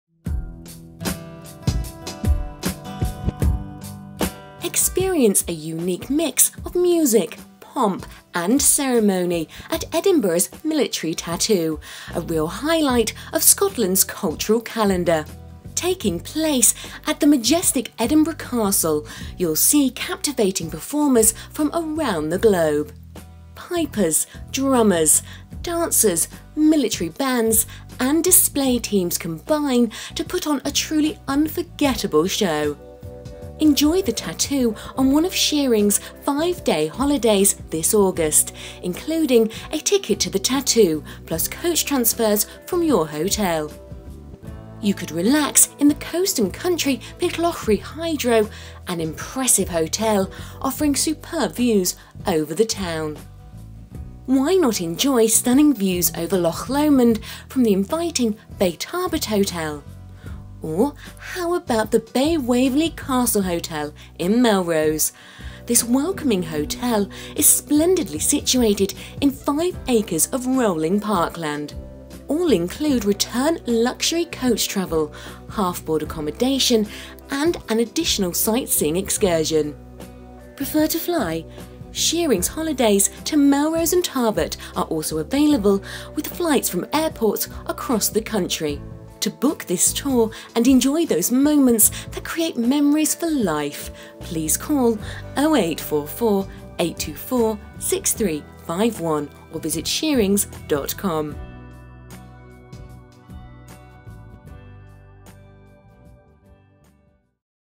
Coach Holiday to the Edinburgh Tattoo
The Royal Edinburgh Military Tattoo is one of the World's most spectacular shows. Set against the inspiring backdrop of the Scottish capital's ancient Castle, this internationally acclaimed event features almost 1000 musicians, pipers, drummers, singers and dancers. Come and explore this fantastic holiday with Shearings Holidays